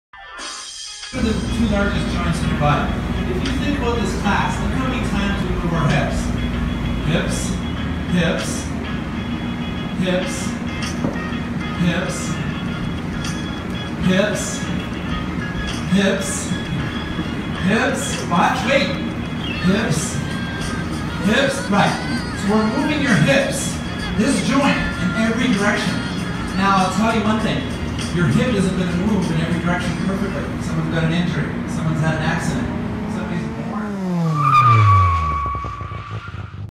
Its All About Your Hips ...
The two largest joints in your body are your. 😊 and it's amazing how many different positions we move, work and adjust our hips in the bikram yoga class. (Try to come up with the answer, and post below to win an entry to the next B~Fire class closest to you)